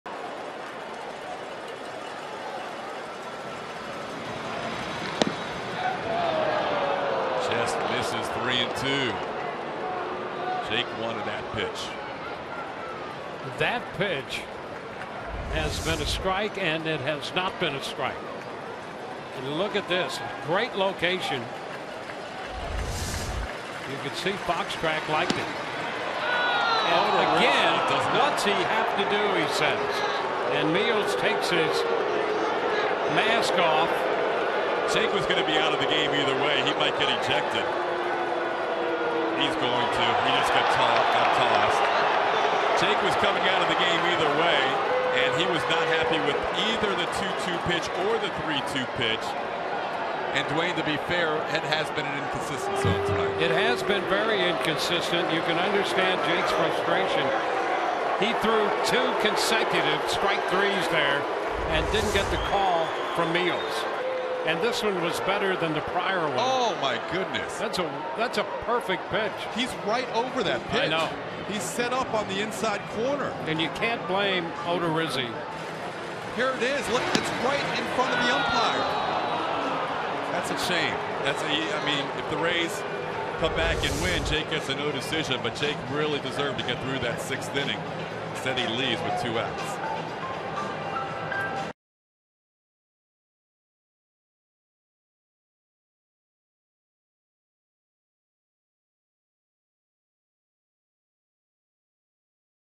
Odorrizi argues with umpire, gets ejected
Jake Odorizzi issues a walk and begins arguing balls and strikes before being ejected by umpire Jerry Meals in the 5th inning